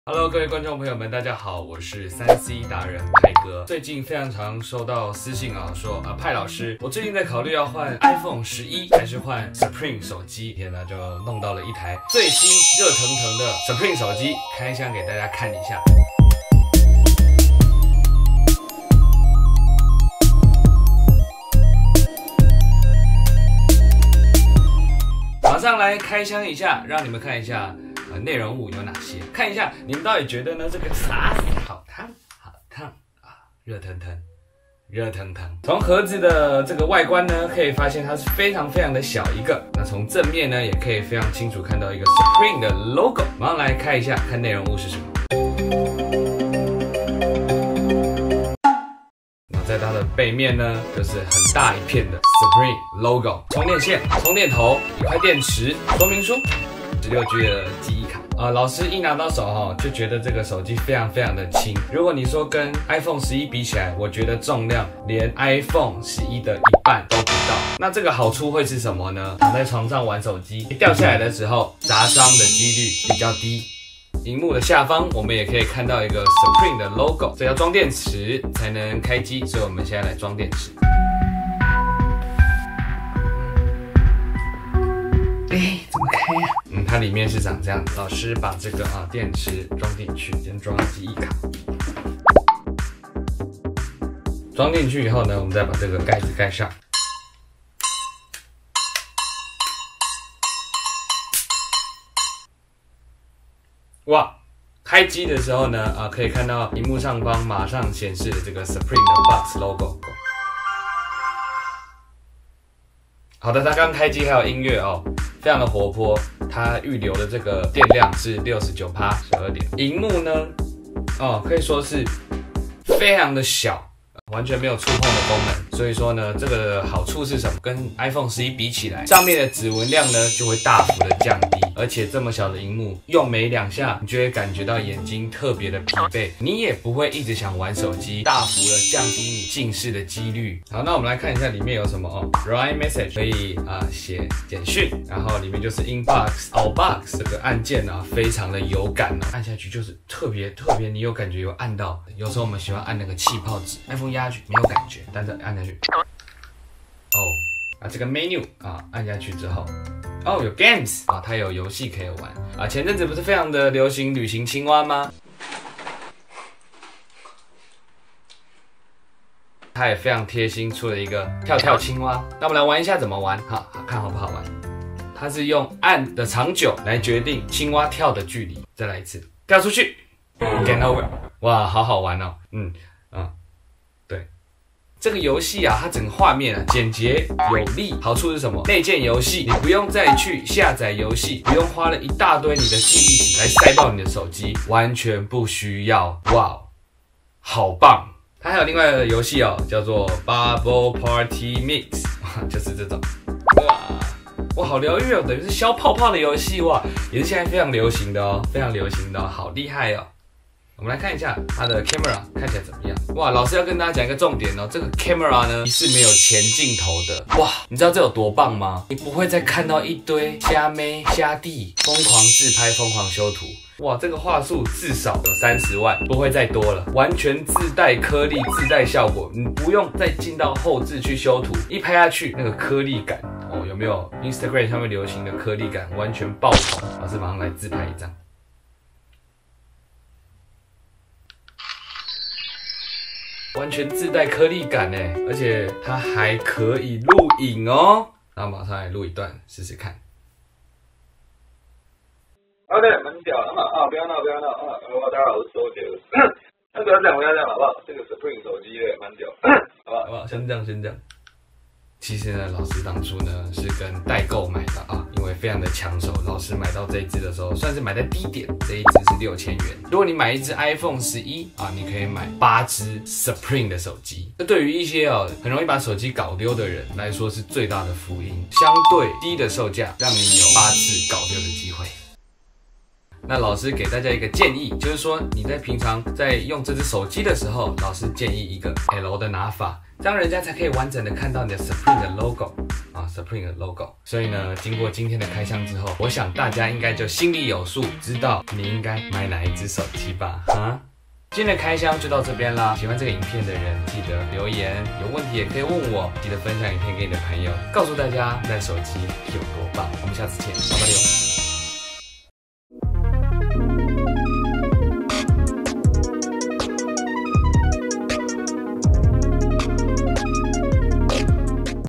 《派開箱》Supreme手機 大勝 IPhone 11？都不用考慮了！超強功能一次公開！
自帶顆粒感鏡頭、減少近視機率！超強功能只有Supreme 有！
每星期三跟某一天都有新影片『 派好笑＆派Vlog＆派開箱』